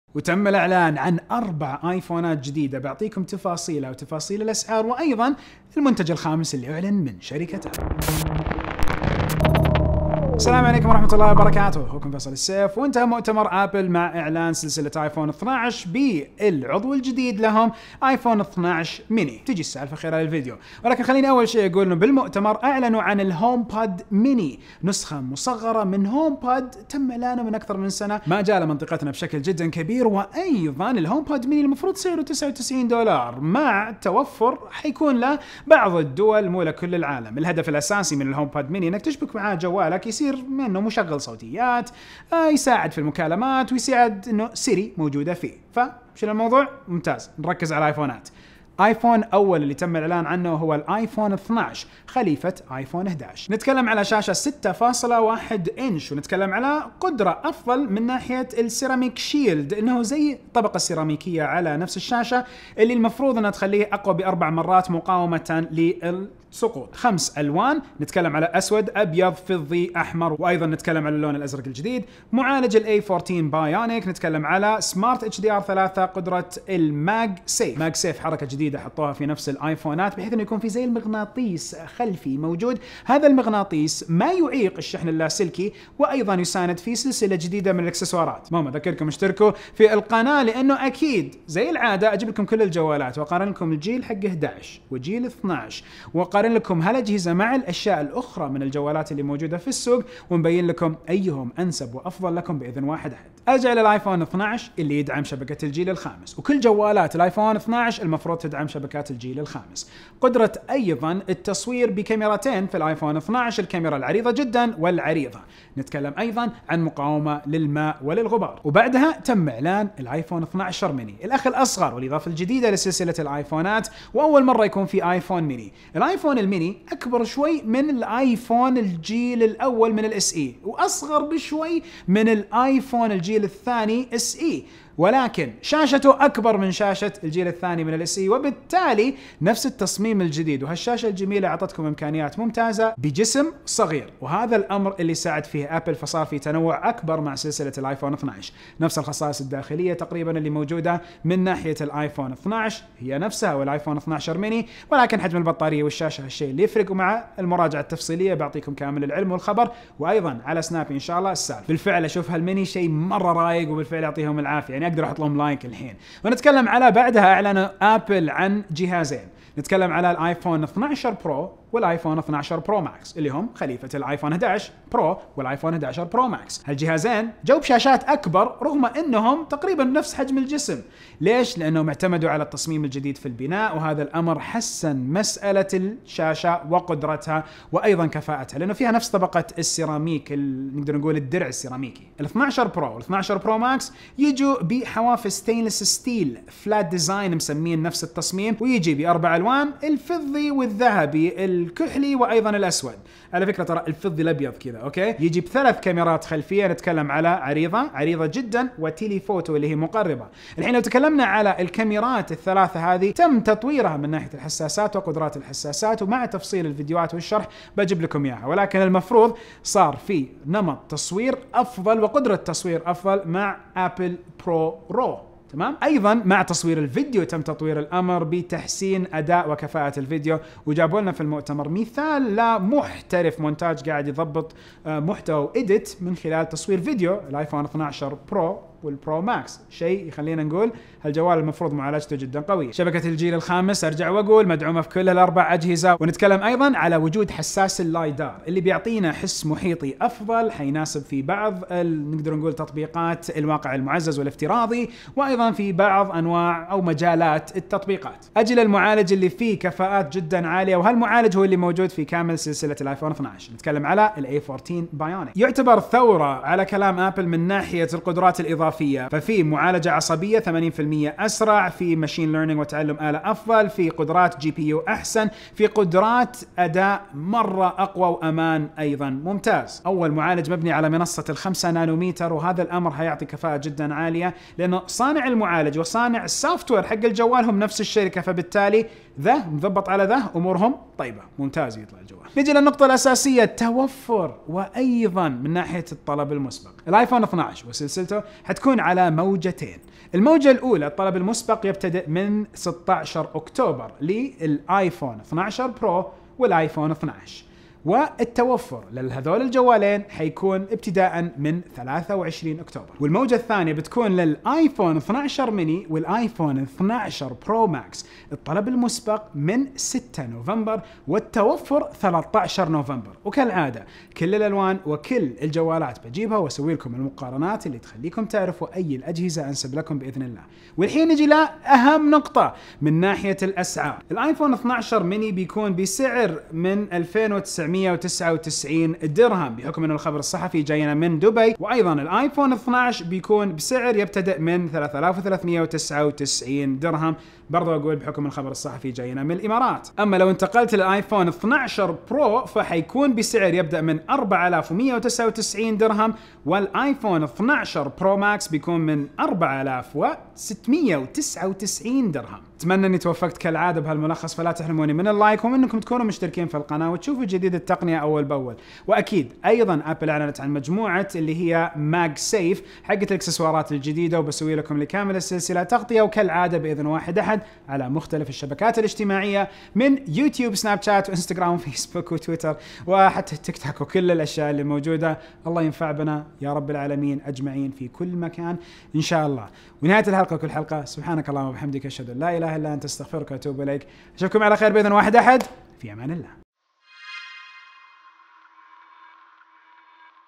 الاعلان رسمياً عن الايفون 12 و منتجات اخرى.. ملخص مؤتمر ابل 2020 !
If the description appears in English that’s due to the settings of your device.